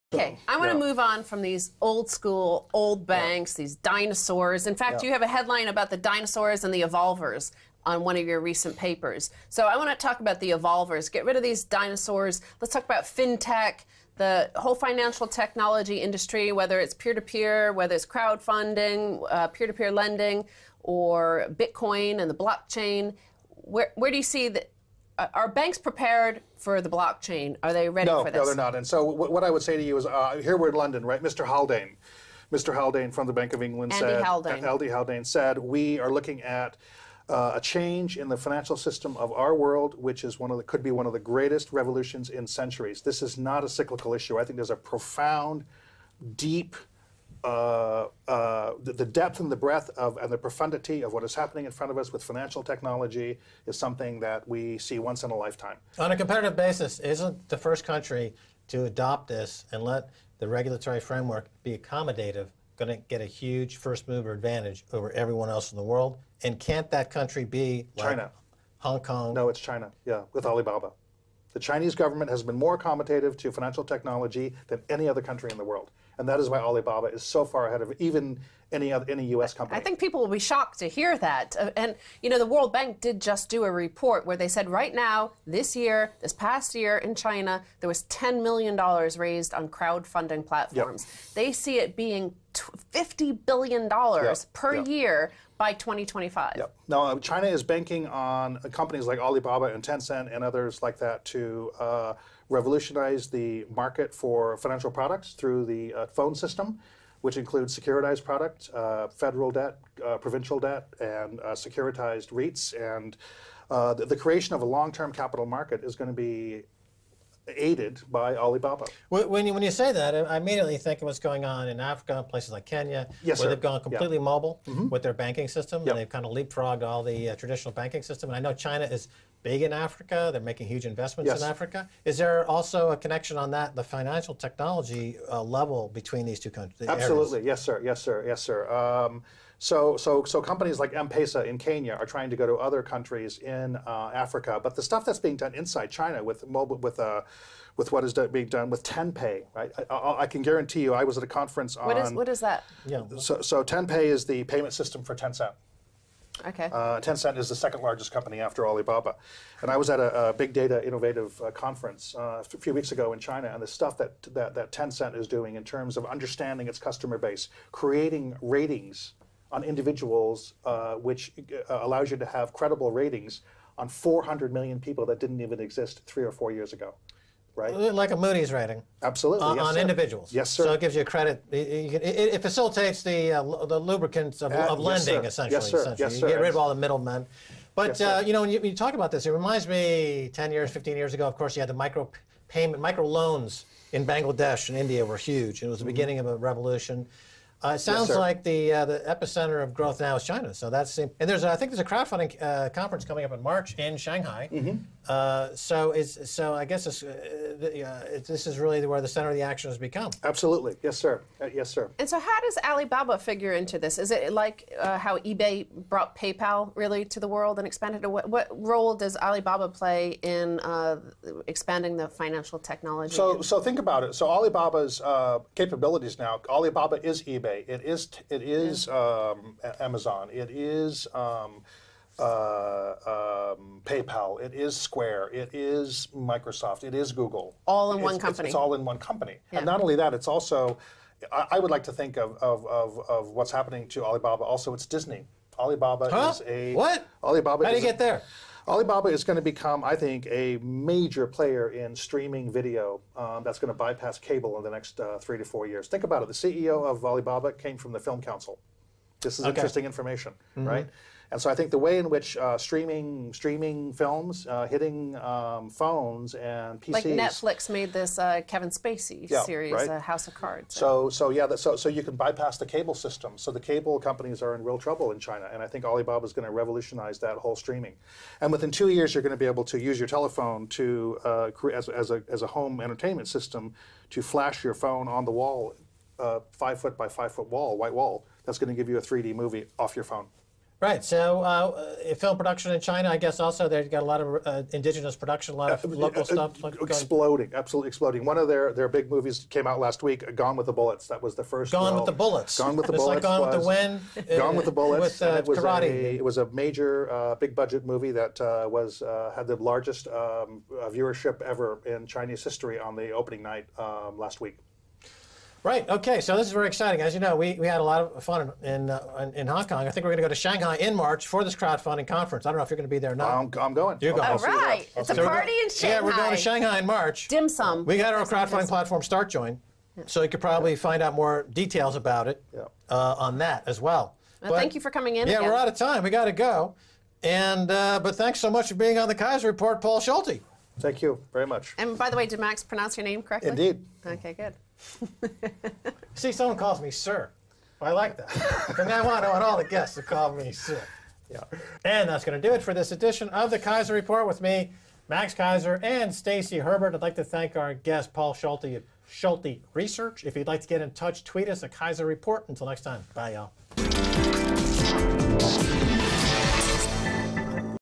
Alibaba will revolutionize world finance
Great Interview of Paul Schulte talking about how China is leapfrogging the global financial model.